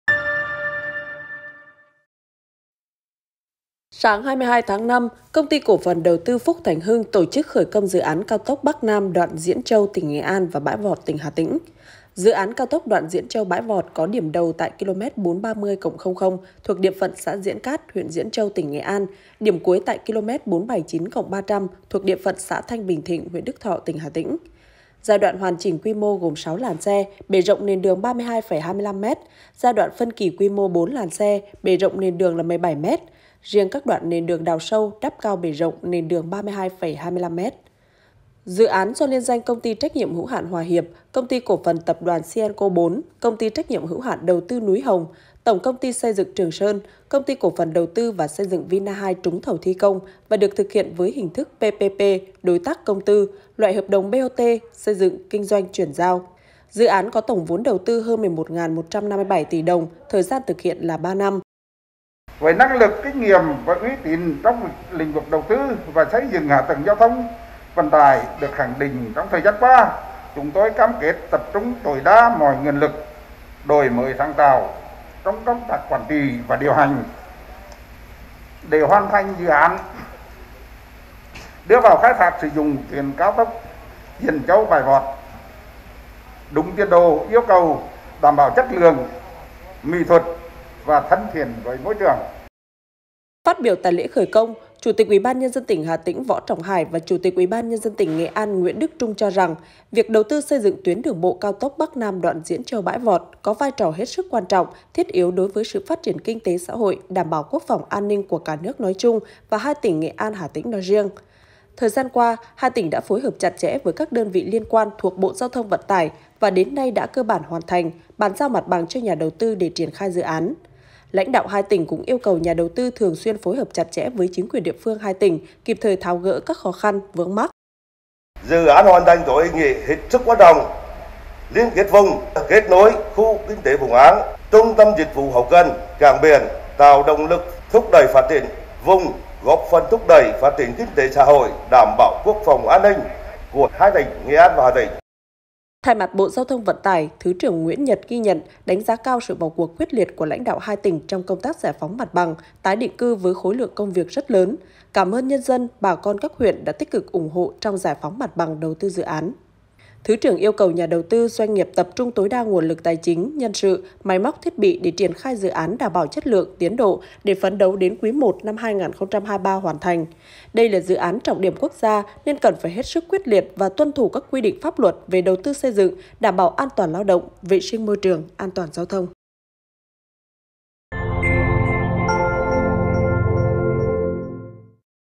Khởi công Dự án cao tốc Diễn Châu (Nghệ An) - Bãi Vọt (Hà Tĩnh)
Ngày 22/5/2021, UBND tỉnh Nghệ An và Hà Tĩnh phổi hợp tổ chức lễ khởi công dự án cao tốc Bắc - Nam đoạn Diễn Châu (Nghệ An) - Bãi Vọt (Hà Tĩnh)

Báo Hà Tĩnh - Tin tức Hà Tĩnh cập nhật 24h liên tục